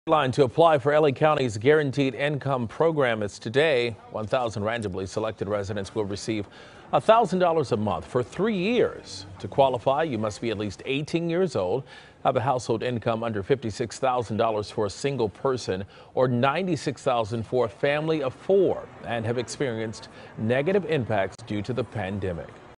Today is the deadline to apply for LA County's guaranteed income program
To qualify, applicants should be at least 18 years old, have a household income under $56,000 for a single person, or $96,000 for a family of four, and have experienced negative impacts from the pandemic. DeMarco Morgan reports.